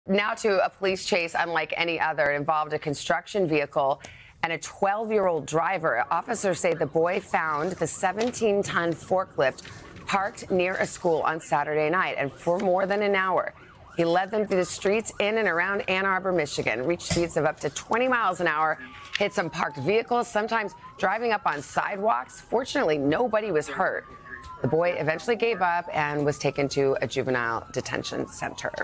12-year-old steals forklift, leads police on hour-long chase
Police say a 12-year-old boy stole a forklift and led them through the streets around Ann Arbor, Michigan, for more than an hour reaching speeds up to 20 miles an hour.